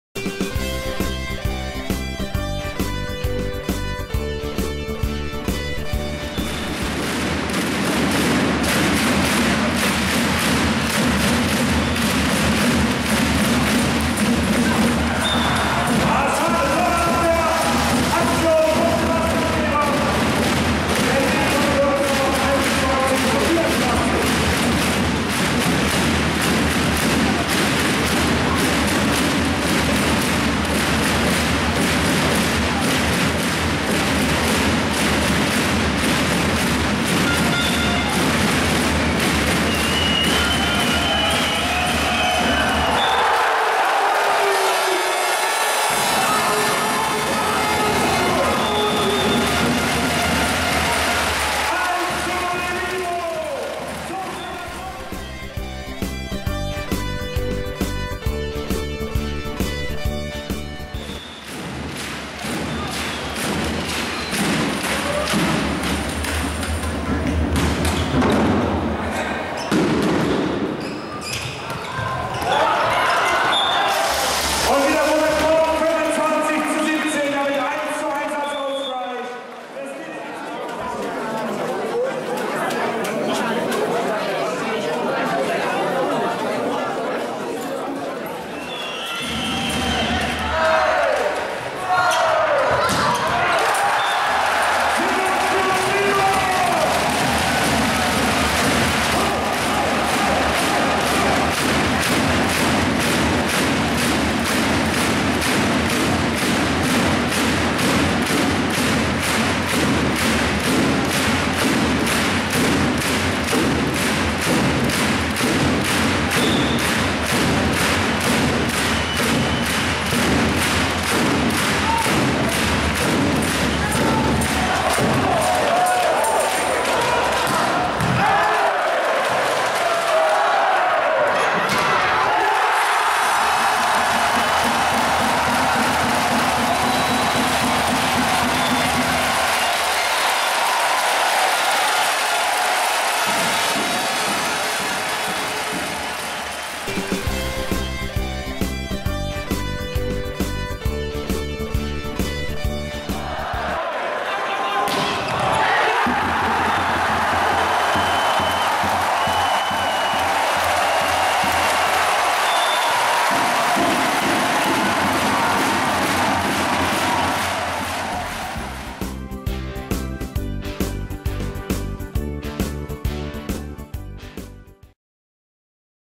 evivo Düren : Berlin Recycling Volleys 3:1
Volleyball-Bundesligist evivo Düren schlägt zu Hause den amtierenden deutschen Meister aus Berlin mit 3:1.
Über 2100 Zuschauer machten die Arena Kreis Düren zu einem Tollhaus!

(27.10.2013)